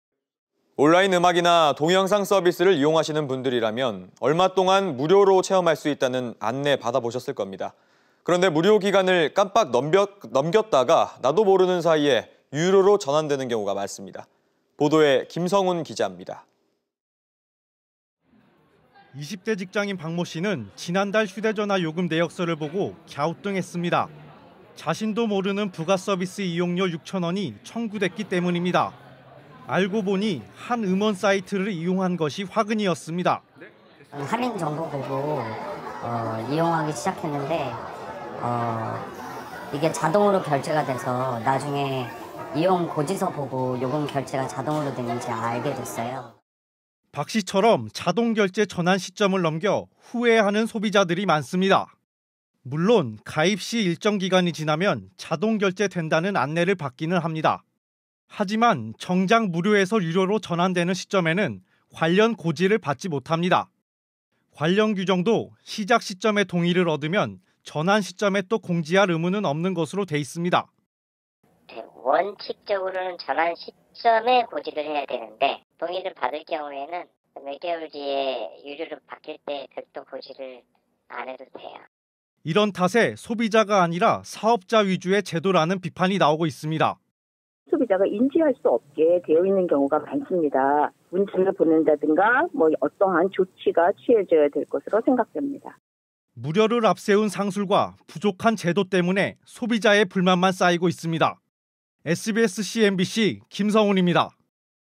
무료체험 한번 받아봤을 뿐인데…‘자동결제’ 상술에 소비자 불만
온라인 음악이나 동영상 서비스를 이용하시는 분들이라면, 얼마 동안 무료로 체험할 수 있다는 안내, 받아보셨을 겁니다.

그런데 무료 기간을 깜빡 넘겼다가 나도 모르는 사이에 유료로 전환되는 경우가 많습니다.

보도에 김성훈 기자입니다.

◇뉴스프리즘 (월~금 저녁 7시~7시 30분,앵커: 김성현, 윤선영)
◇출연: 김성훈 기자